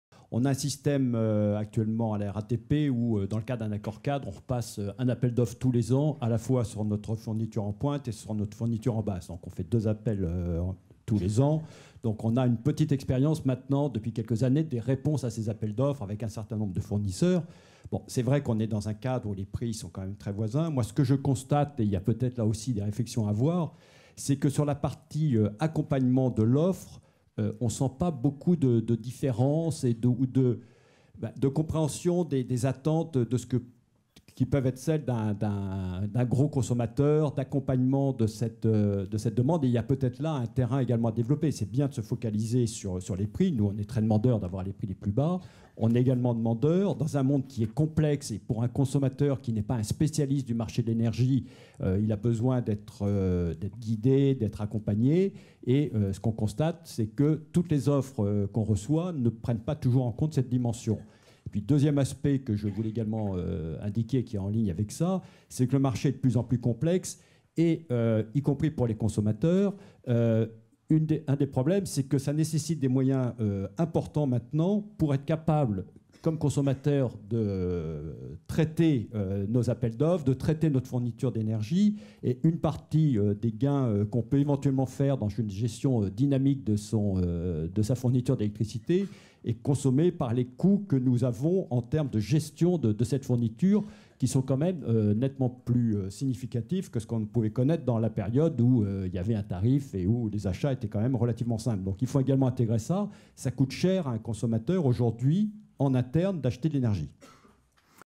Dur, dur, la vie d'acheteur d'électricité - Eric Dyèvre
Ancien commissaire à la Commission de régulation de l'énergie (CRE), Eric Dyèvre a découvert à la RATP, dont il est le directeur du département gestion des infrastructures, la vie d'acheteur d'électricité...